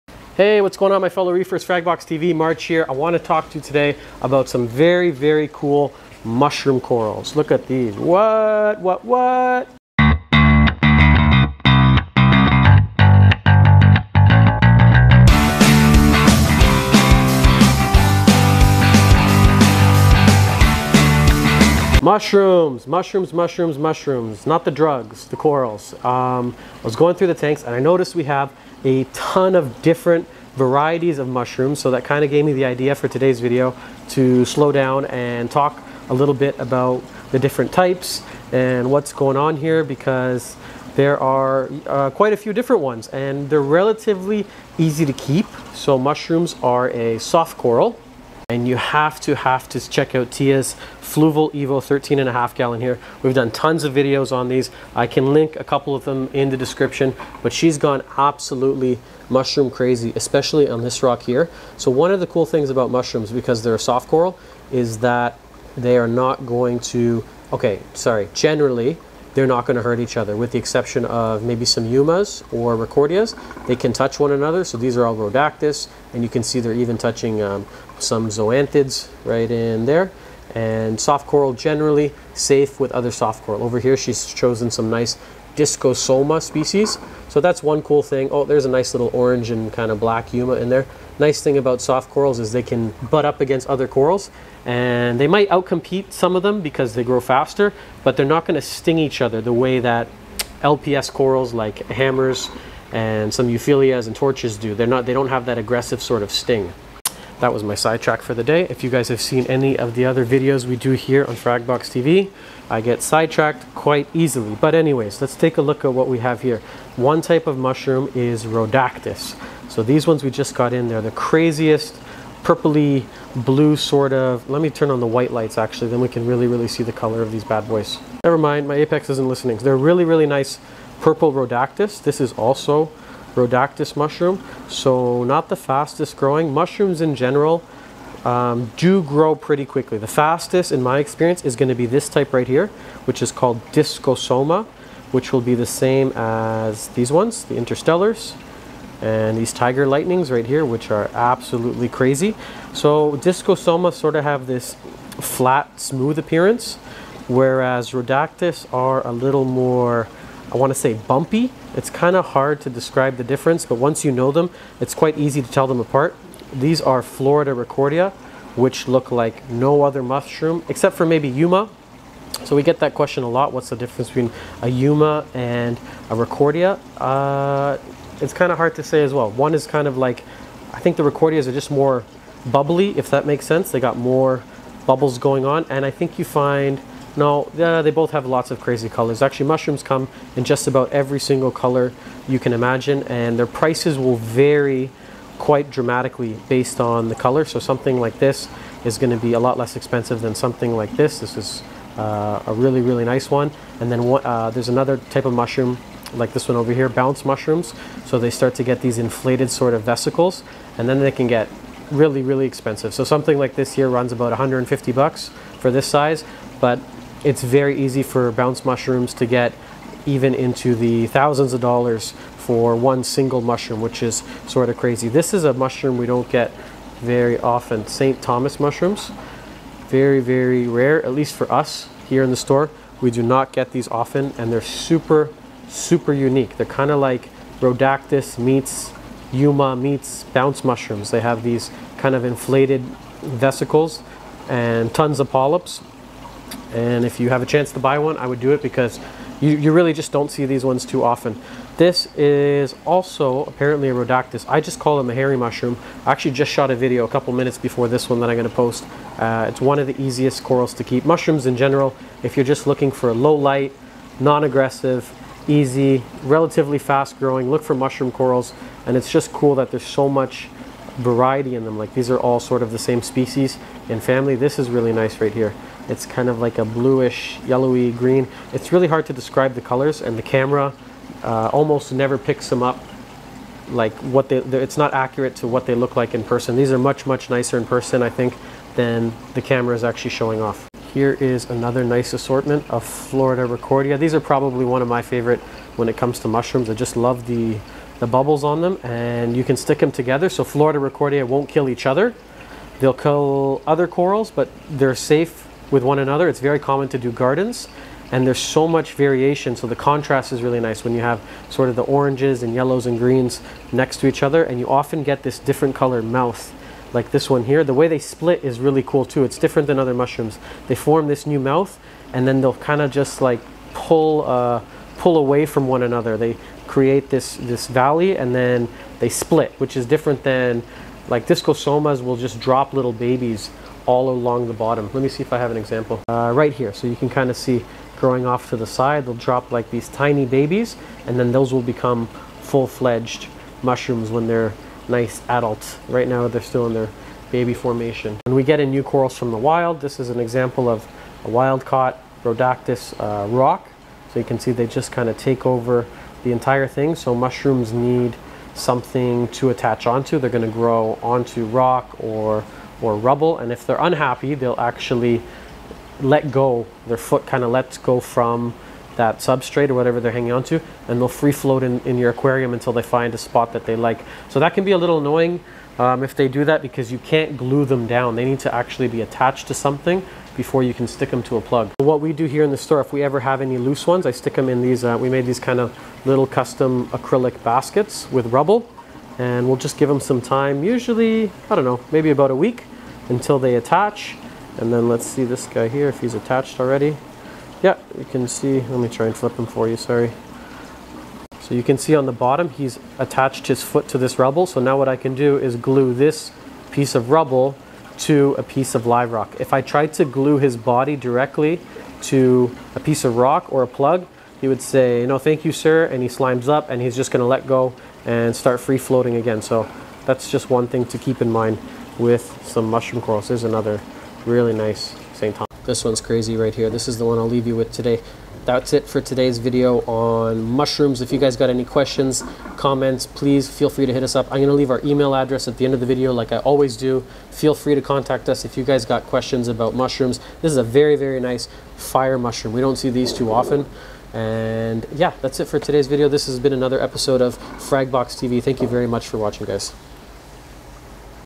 Keeping Mushroom Corals \\ One Of The Easiest Corals Ever
We had so many different types in stock which doesn't normally happen, it felt like we had to do a video to show the differences
You can find all our available corals here